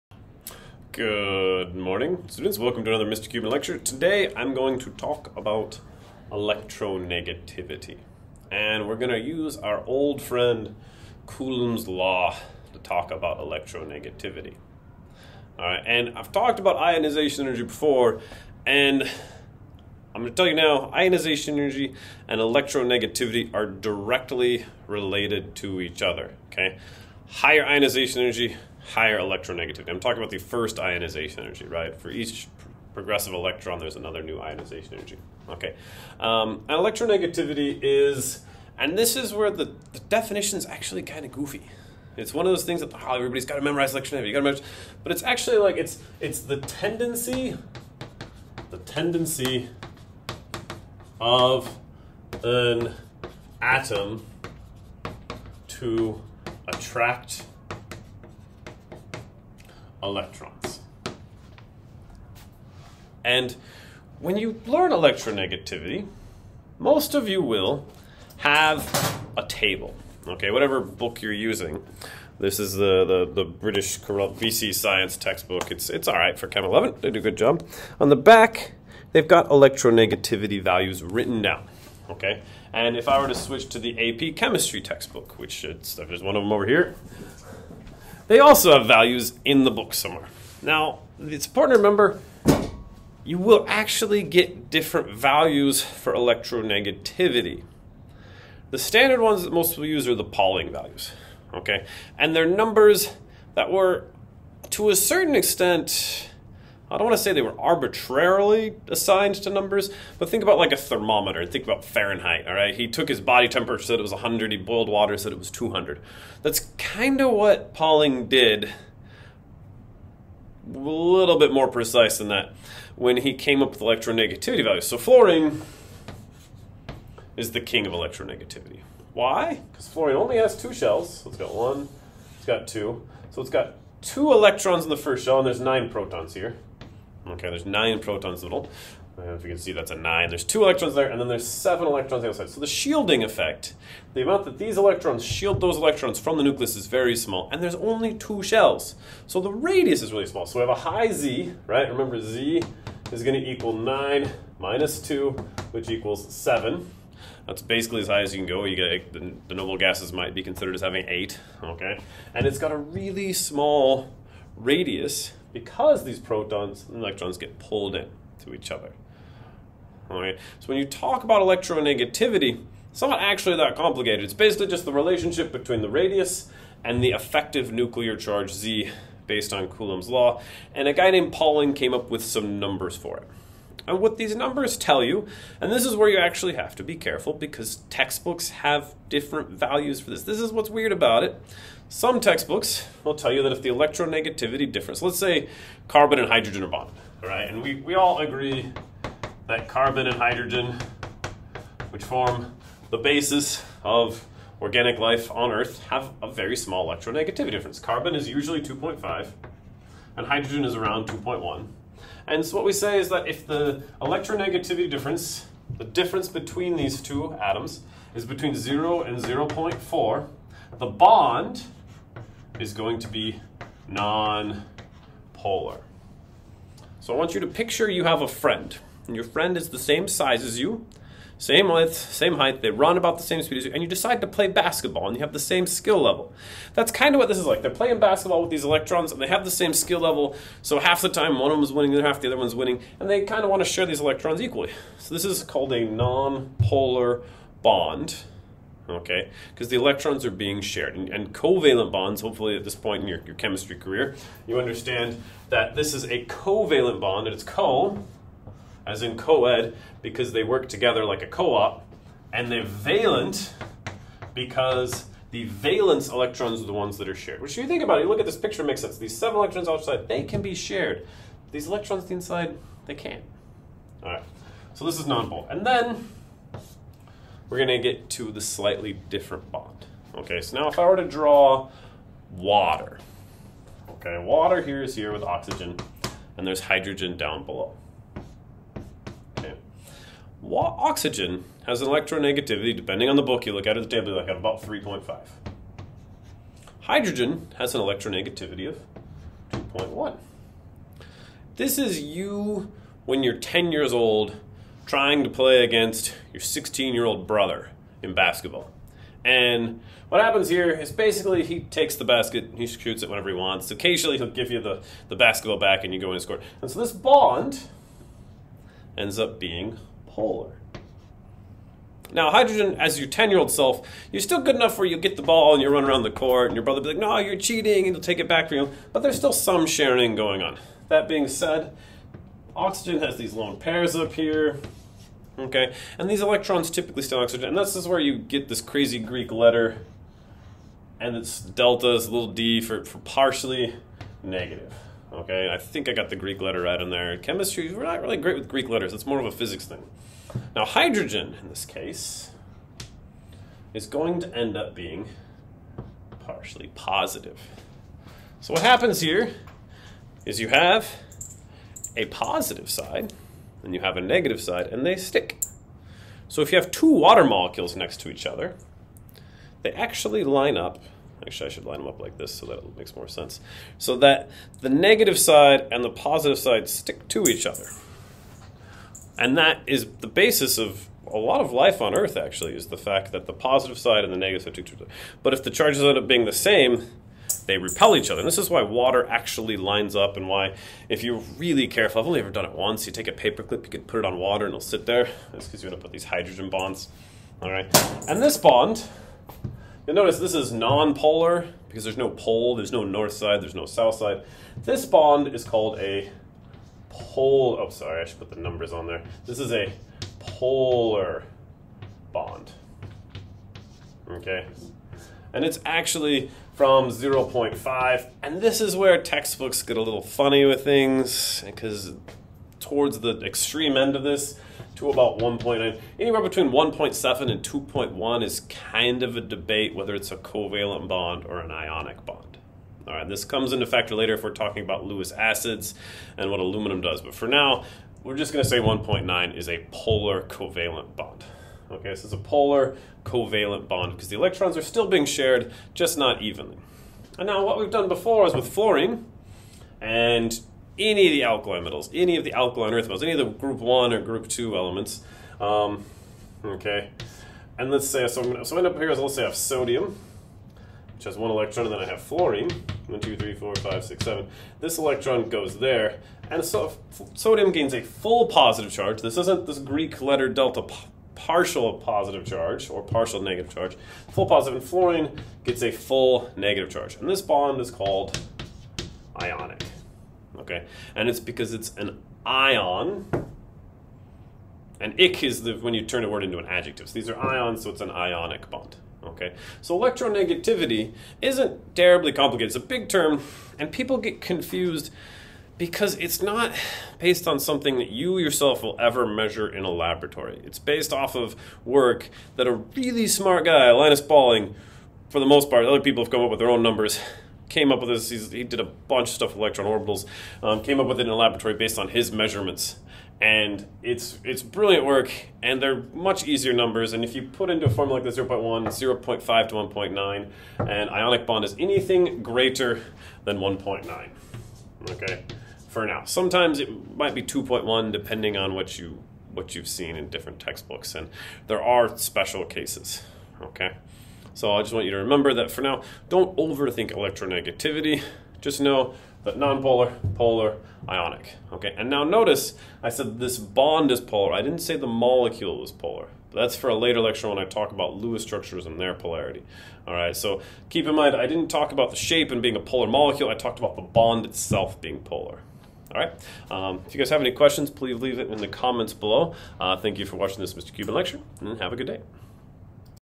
electronegativity and polar vs non polar bond
This video talks about electronegativity, defines the term. Explains pauling electronegativity and then goes in-depth on a few special cases with bonds.